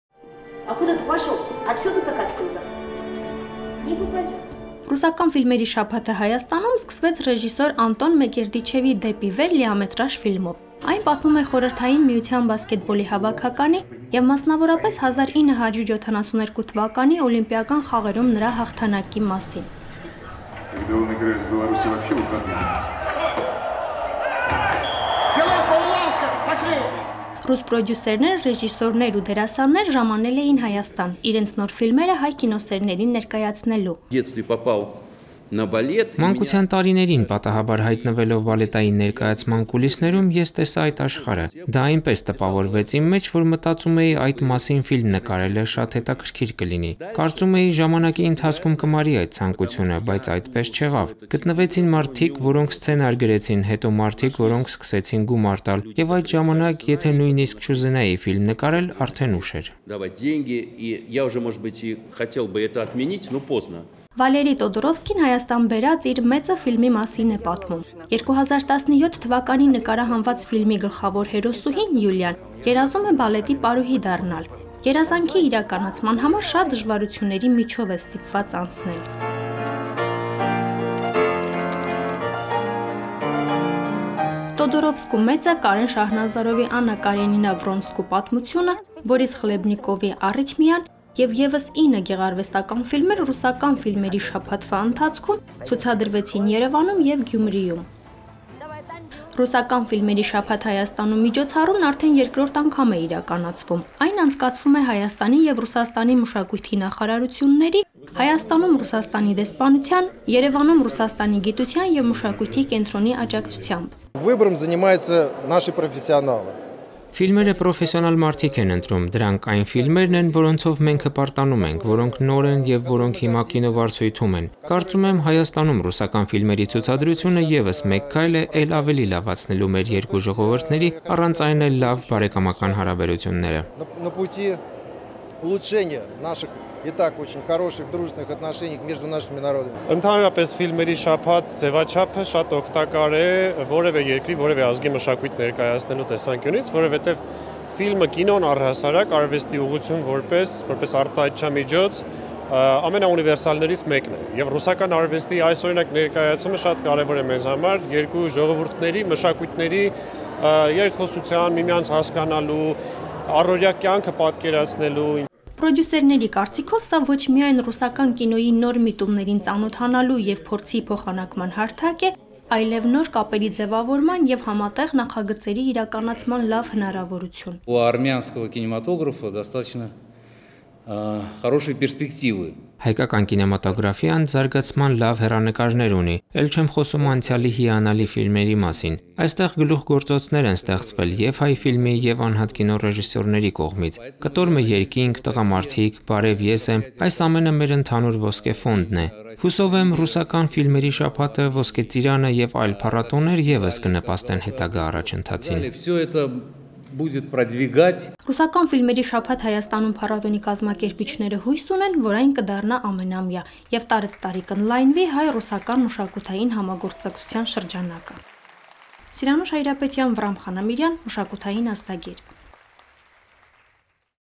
Հայաստանում ռուսական ֆիլմերի շաբաթ էր
Մայիսի 16-ից 20-ը Հայաստանում ռուսական ֆիլմերի շաբաթ էր: Շաբաթն սկսվեց ռեժիսոր Անտոն Մեգերդիչևի «Դեպի վեր» լիամետրաժ ֆիլմով: Այն պատմում է Խորհրդային Միության բասկետբոլի հավաքականի և 1972 թ. Օլիմպիական խաղերում  նրա հաղթանակի մասին: